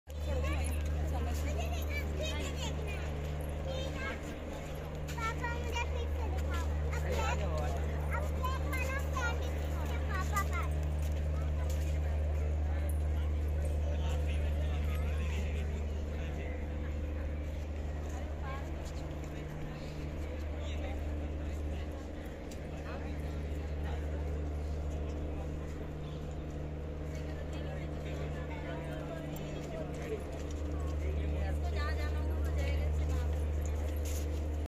Fearless Deer Playing in the Zoo - Must Watch! zoo india //National zoo
The specific name commemorates the French naturalist Alfred Duvaucel.[3]

The swamp deer differs from all other Indian deer species in that the antlers carry more than three tines. Because of this distinctive character it is designated bārah-singgā, meaning "twelve-horned" in Hindi.[4] Mature stags usually have 10 to 14 tines, and some have been known to have up to 20.[
Barsingha deer##
Barsingha deer#